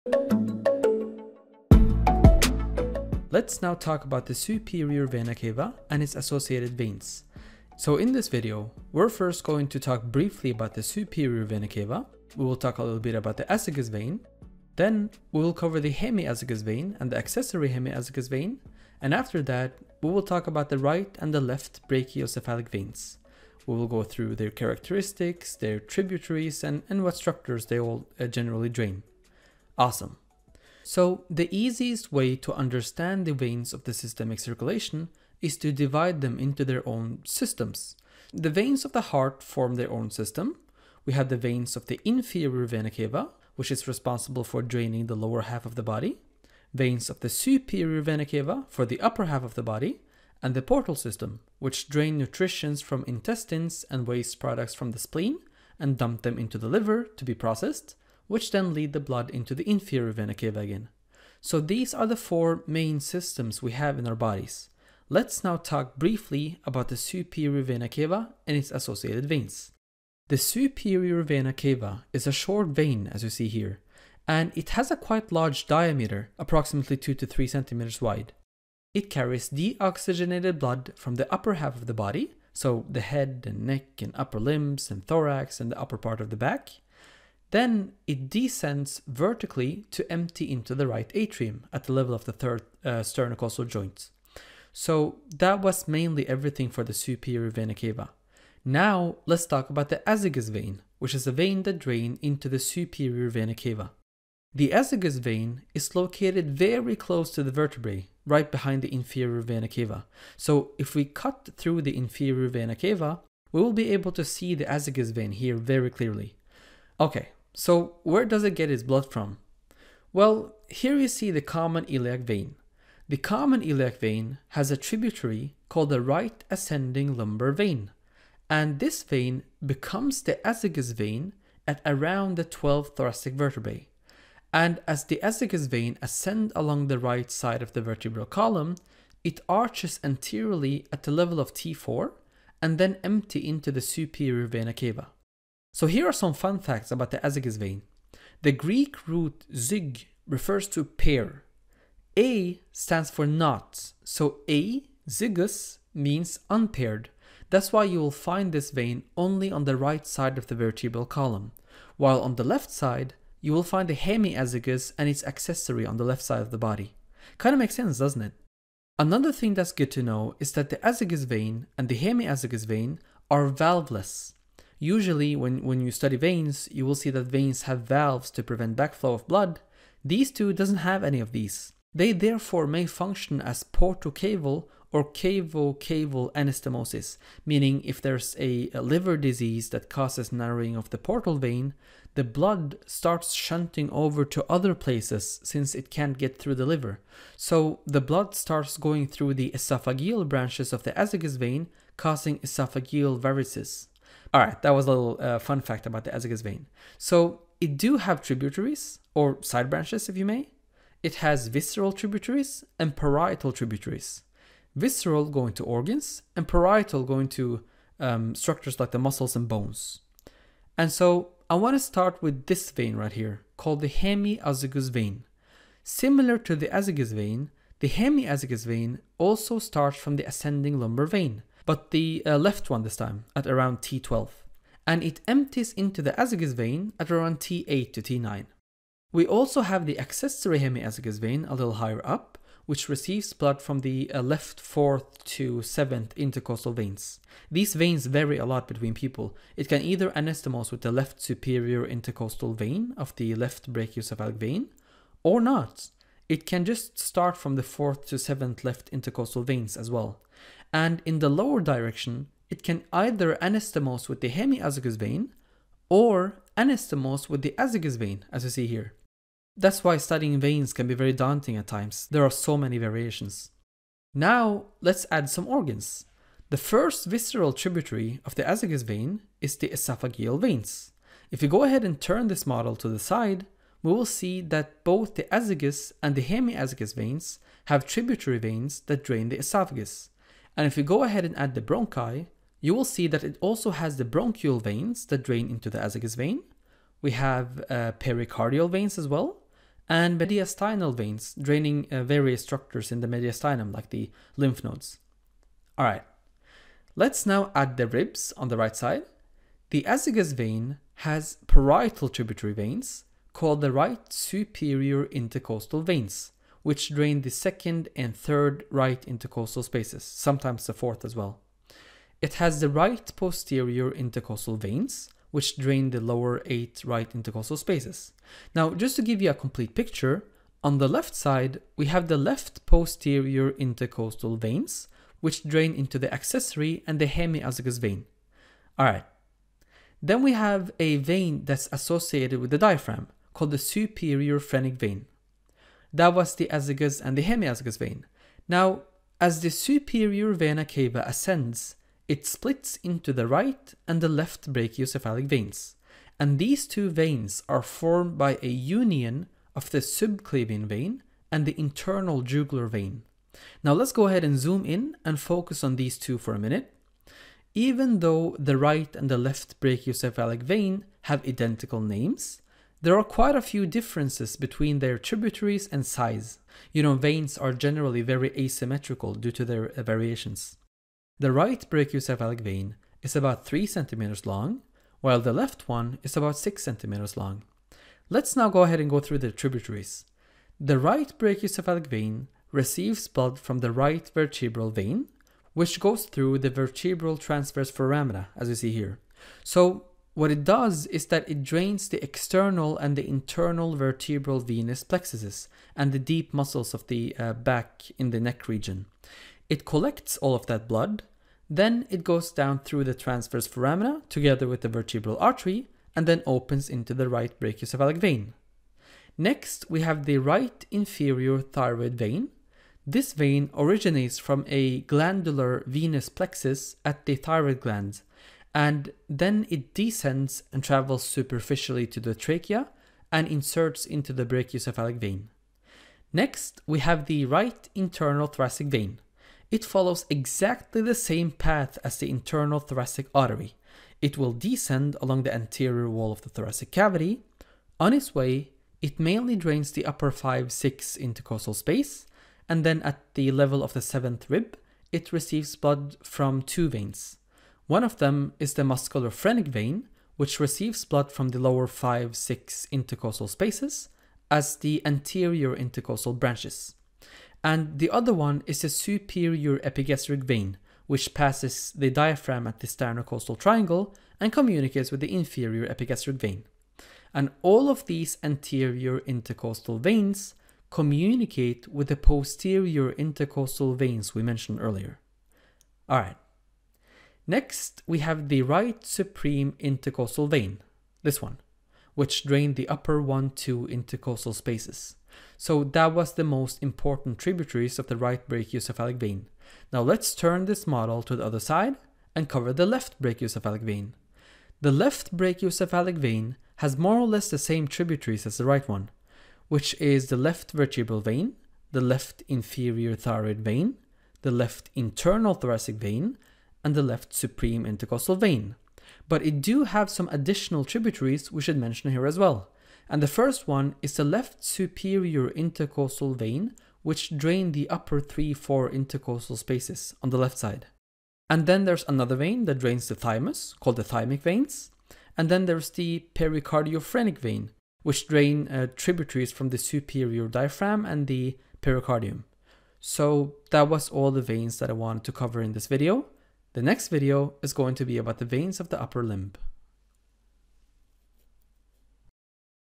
Superior Vena Cava Tributaries (Azygos, Hemiazygos, Brachiocephalic) - Anatomy
Content:
0:00 Introduction
00:29 Veins of the Systemic Circulation
01:05 Superior Vena Cava
01:32 Azygos & Hemiazygos Vein
05:45 Brachiocephalic Veins
06:20 Right Brachiocephalic Vein
08:19 Left Brachiocephalic Vein

Superior Vena Cava
 - Short Vein
 - Large Diameter: 2-3 Cm
 - No valves

Azygos Vein (vena azygos)
 - Unpaired vein located behind the Inferior Vena Cava, on the right side of the vertebral column.
 - Received blood from the Right Ascending Lumbar Vein (v. Lumbalis ascendens dexter)
 - May function as portocaval and cavo-caval anastemoses
Tributaries:
 - Visceral Tributaries
  ○ Esophageal Veins (vv. oesophageales)
  ○ Bronchial Veins (Vv. bronchiales)
  ○ Pericardial Veins (Vv. Pericardiacae)
  ○ Mediastinal Veins (Vv. Mediastinales)
 - Parietal Tributaries
  ○ Hemiazygos Vein (vena hemiazygos)
    □ Acessory Hemiazygos Vein (vena hemiazygos accessoria)
  ○ Right Superior Intercostal Veins (Vv. Intercostales Superior Dextrae)
  ○ Right Posterior Intercostal Veins (Vv. Intercostales Posterior Dextrae)
  ○ Superiro Phrenic Vein (Vv. Phrenicae SUperiores)

Left Thoracic Veins:
 - Left Posterior Intercostal Veins (Vv. Intercostales Posteriores Sinistrae) draining into the accessory hemiazygos vein and the hemiazygos vein

Right Brachiocephalic Vein (Vena brachiocephalica dextra)
 - Formed by a union of the Subclavian Vein (vena subclavia) and the Internal Jugular Vein (vena jugularis interna)
 - About 3 cm long
 - Right Vertebral Vein (vena vertebralis)
 - Right Inferior Thyroid Vein (vena thyroidea inferior)
 - Right Internal Thoracic Vein (vena thoracica interna dextra)
  ○ Anterior intercostal veins (venae inercostales anteriores) communicate with the posterior intercostal veins
  ○ Musculophrenic Vein (vena musculophrenica)
  ○ Superior Epigastric Vein (vena epigastrica superior)
 - Right Supereme Intercostal Vein (vena intercostalis suprema)

Left Brachiocephalic Vein
 - About 6 cm long
 - Left Vertebral Vein (vena vertebralis dexter)
 - Left Inferior Thyroid Vein (vena thyroidea inferior sinister)
 - Left Internal Thoracic Vein (vena thoracica interna sinister)
 - Left Supreme Intercostal Vein (vena intercostalis suprema sinister)
 - Left Superior Intercostal Vein (vena intercostalis superior sinistra)
 - Thymic Veins (vv. thymicae)
 - Pericardiophrenic Veins (venae percardiophrenicae)